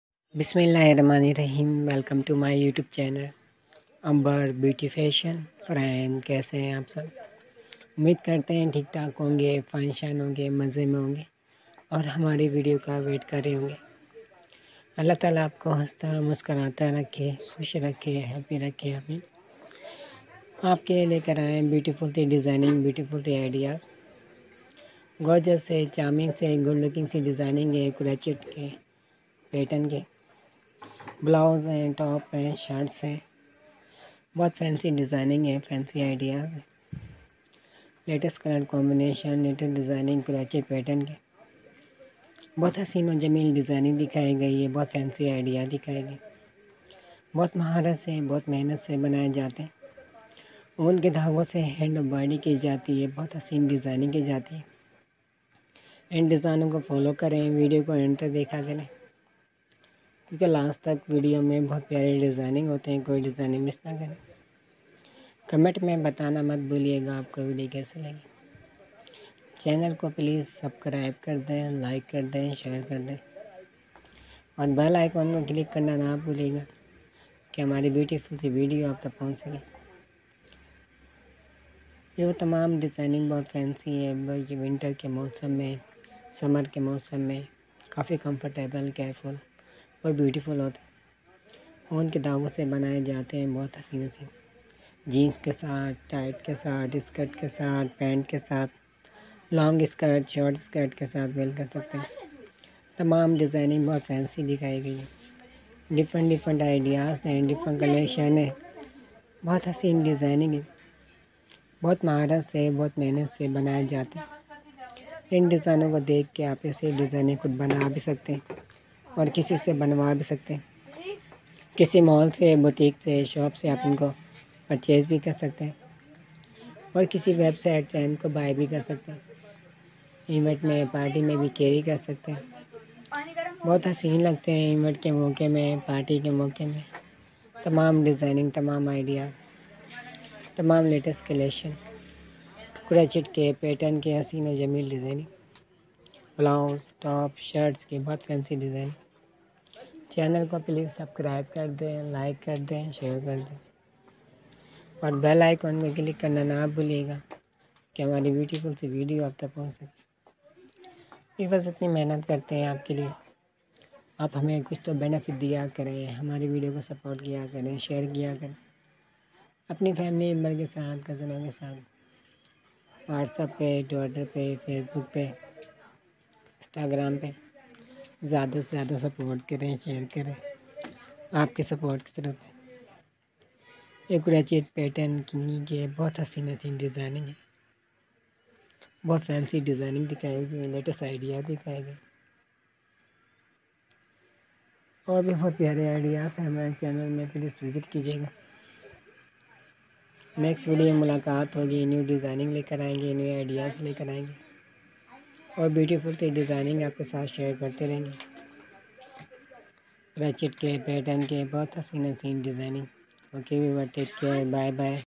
Top 50 Latest stylish trendy easy crochet handknit blouse top pattern designs for woman
is video me ap dakhe ge Top 50 Latest stylish trendy easy crochet handknit blouse top pattern designs for woman